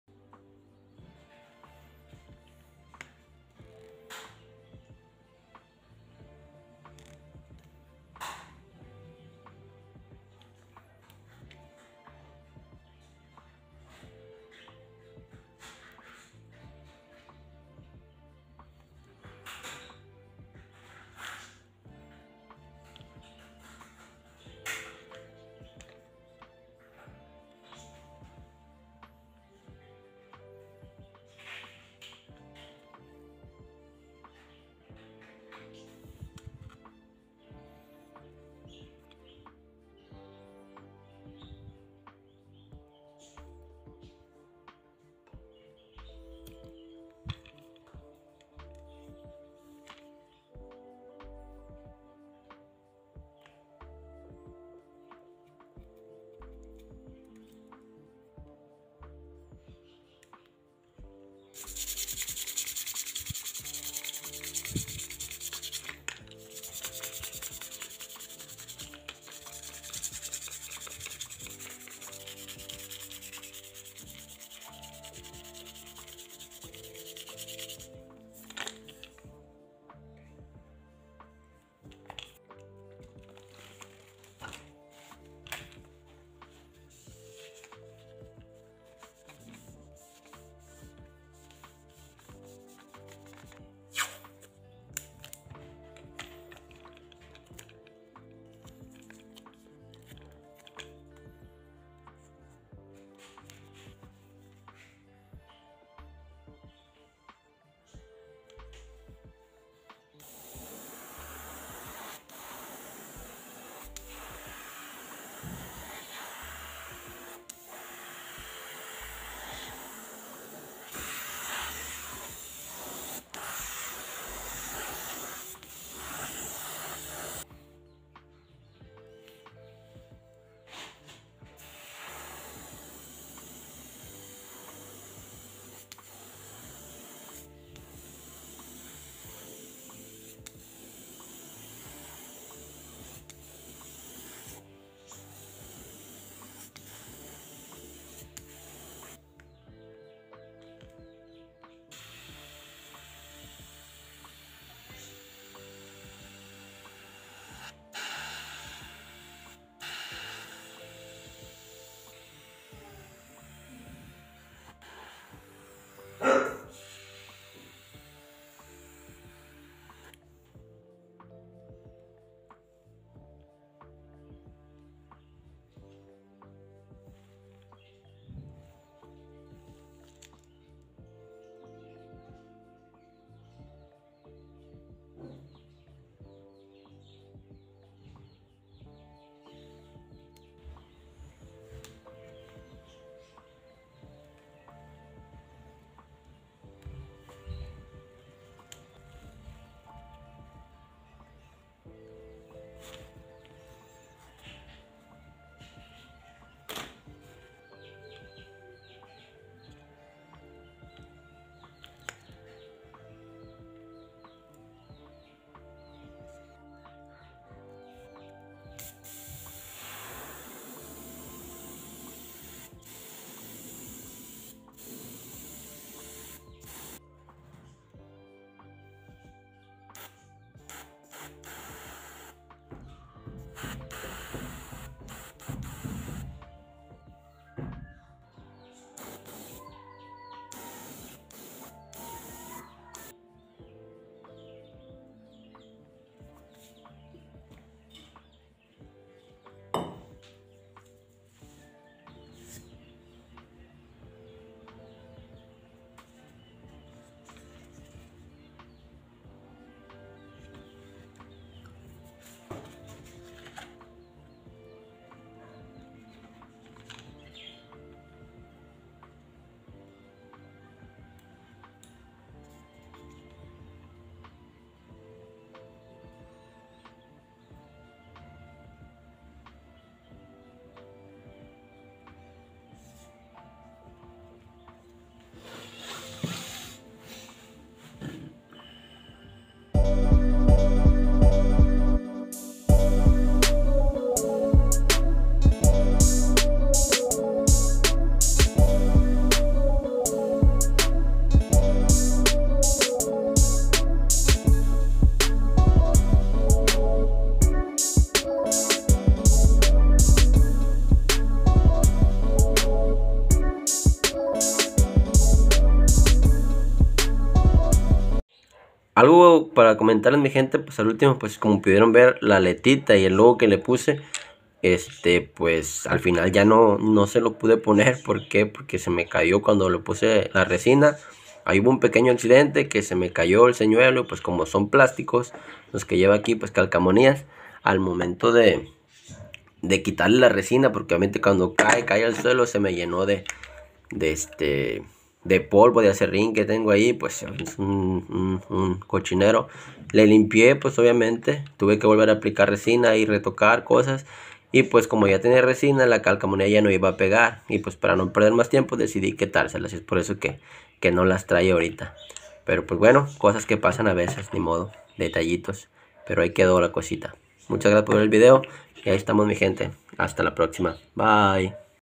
pintando señuelo/ restauracion/ tuneando minnow🤙🤙
que onda banda en este videito solo vamos a pintar este señuelo para darle una segunda oportunidad, muchas gracias por su apoyo:)